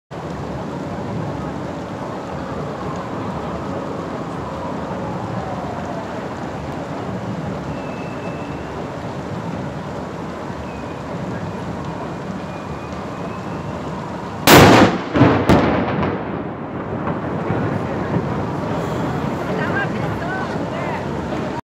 Cañonazo cerro santa lucía
Cañonazo de las 12:00 en cerro santa lucía